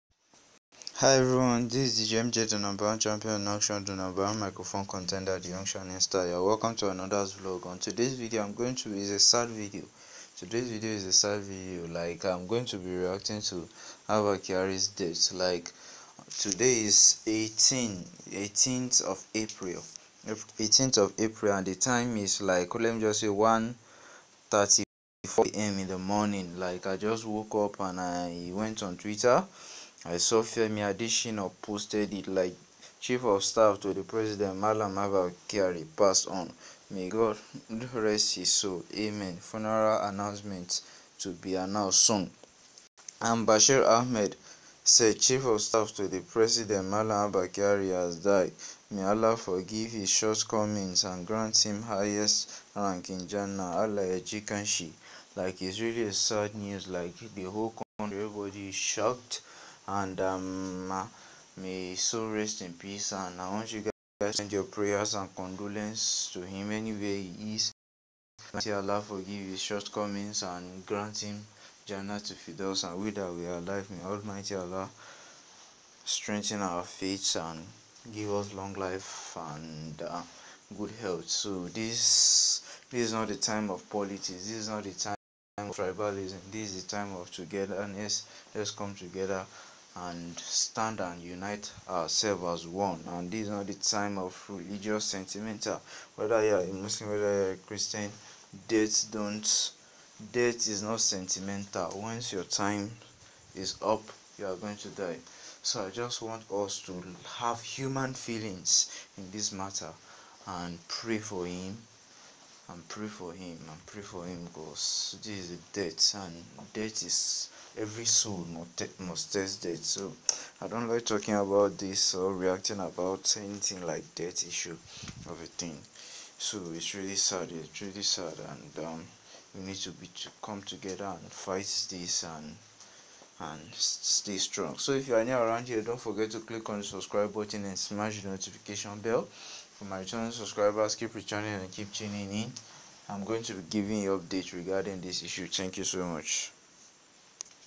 Chief of Staff to the President, Abba Kyari, has died | Abba Kyari, Buhari's Chief of Staff is dead
Chief of Staff to the President, Abba Kyari, has died | Abba Kyari has pass away | Abba Kyari